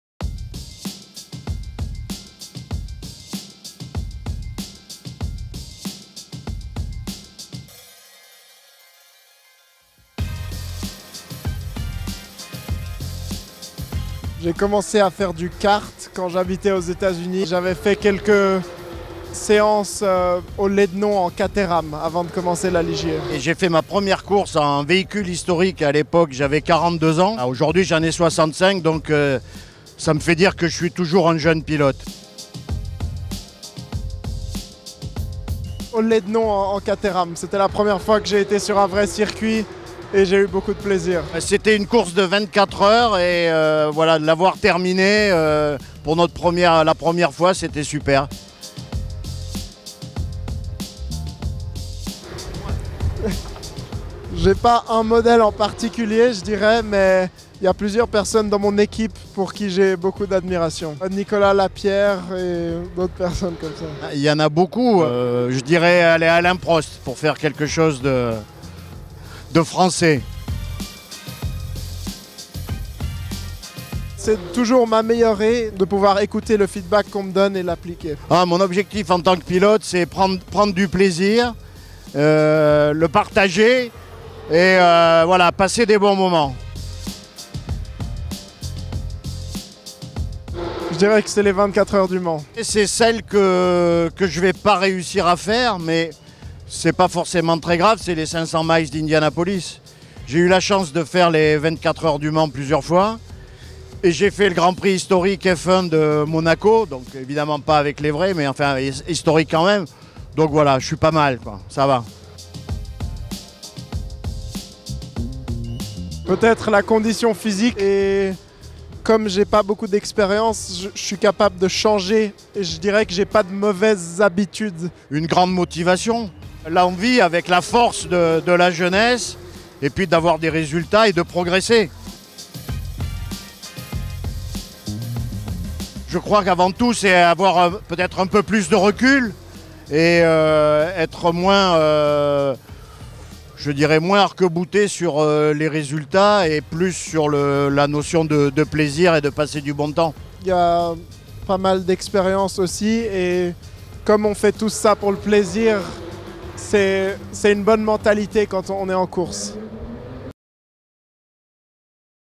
Joint interview with Cédric Oltramare and Jacques Nicolet
The rookie (#4 COOL Racing ligierjs2r) and the gentleman driver (#33 Les Deux Arbres ligierjsp4) of the 2021 season discussed their personal stories in motorsport 🏎️

Get to know more about them 🔍